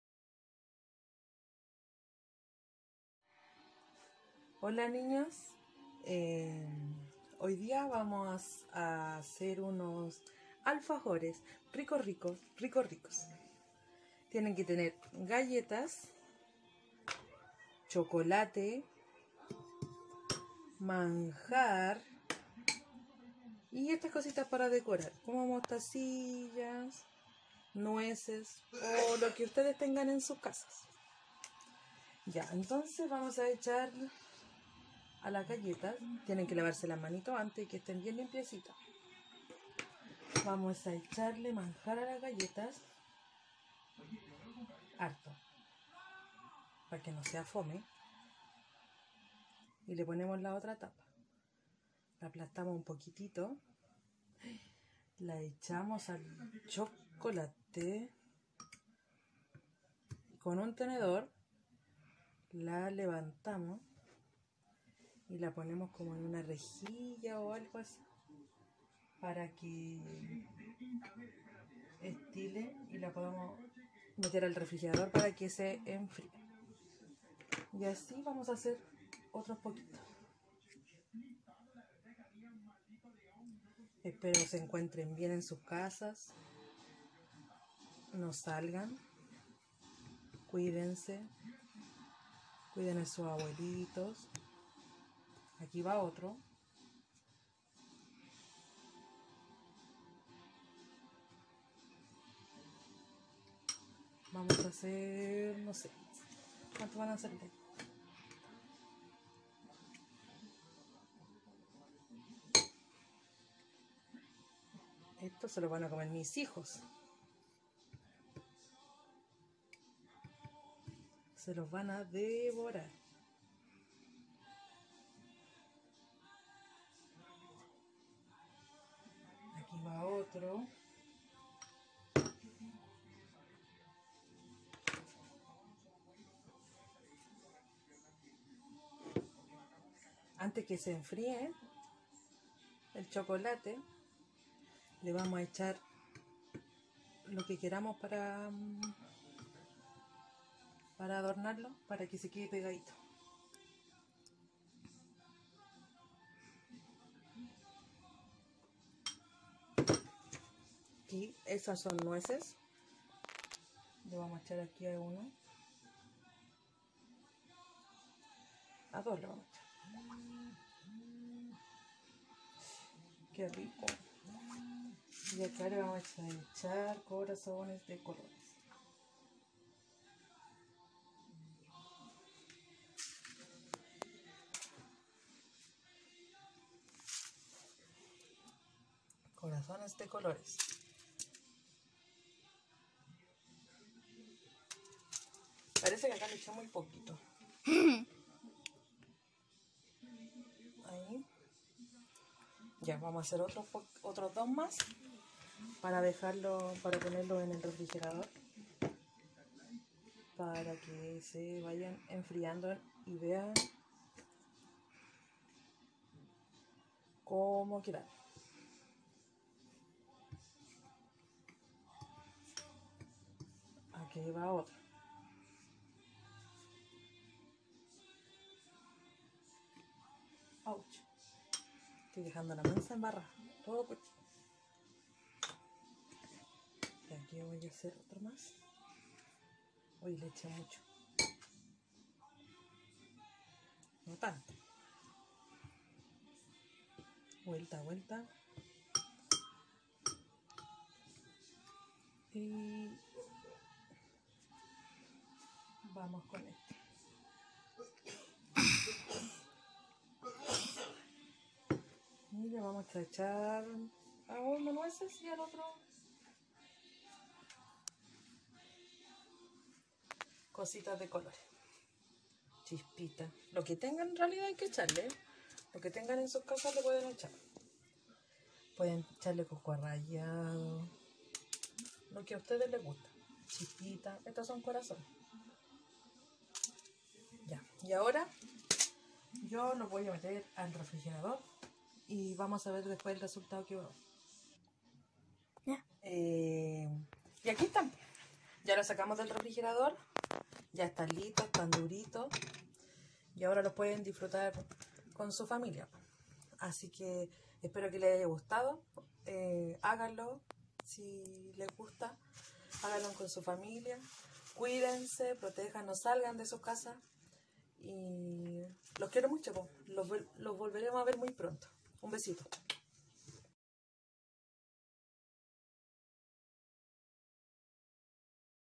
taller de cocina "alfajores" con la tia Cote
Esta actividad se puede hacer en familia